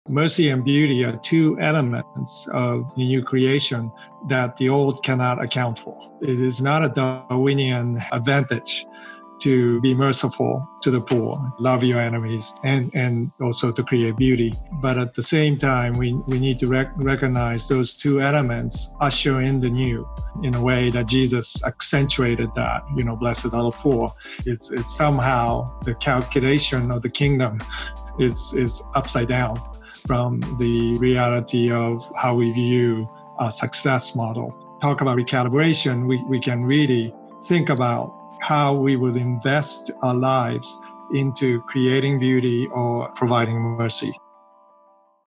It is not a Darwinian advantage to be merciful to the poor | Mako Fujimura
Mako Fujimura (Artist & Author) explores why we must show mercy and beauty towards others.